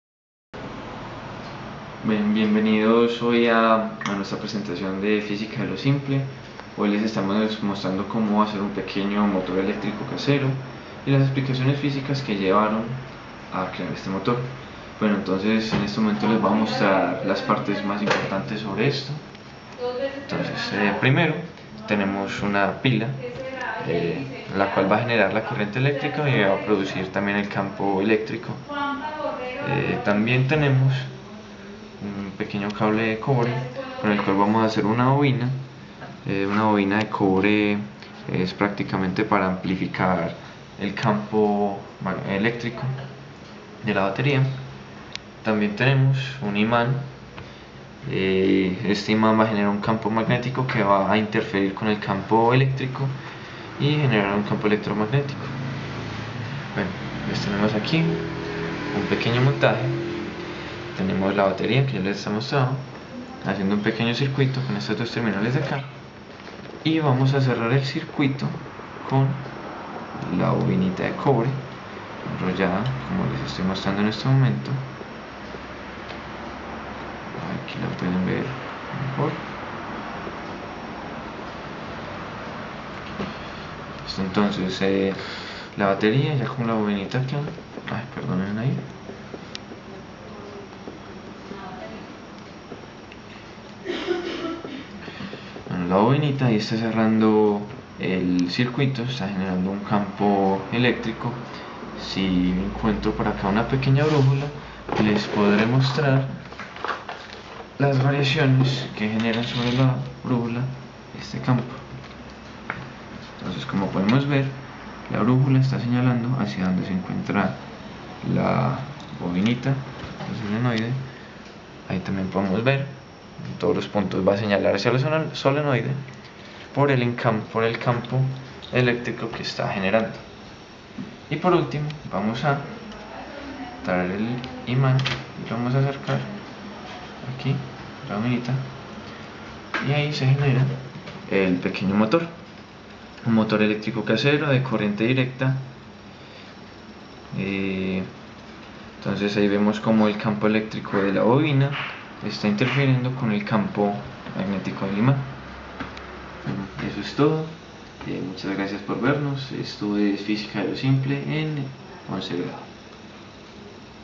Construccion de un Motor Electrico Casero
Pequeño y simple motor electrico de DC con explicacion de los fenomenos fisicos involucrados. Trabajo para el curso de Fisica del Colegio Montessori en el grado 11 en 2011.